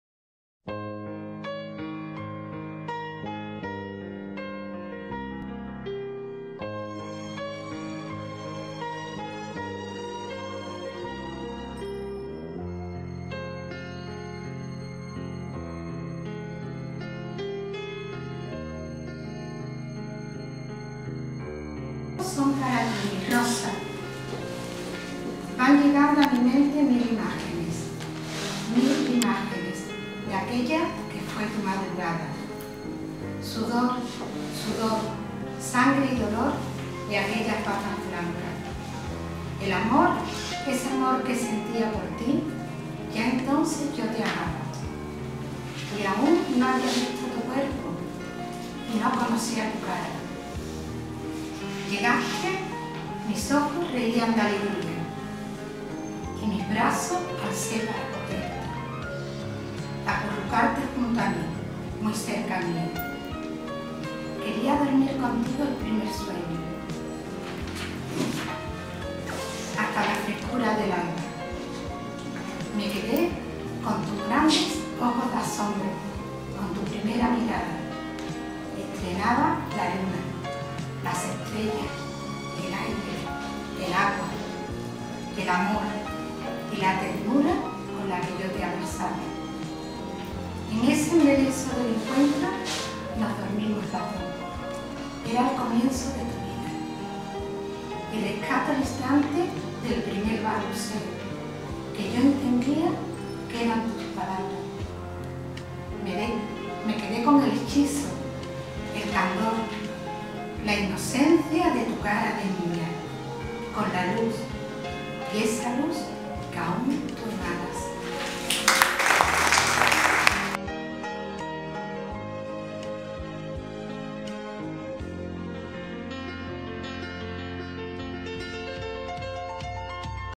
ME QUEDÉ
Un poema de Rosa Pazos Martín. Dedicado a su hija Rosa, donde resalta muy bien lo que se siente al tener un hijo.